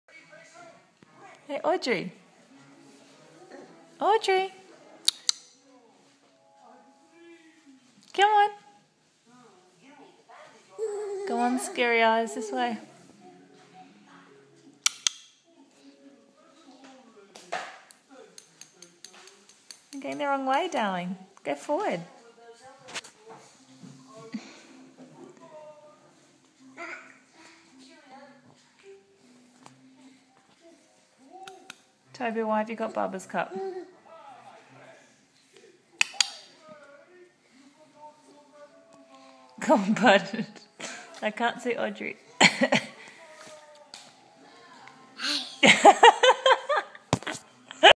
Our crazy son
Trying to get video of Audrey when Toby decides he wants the attention!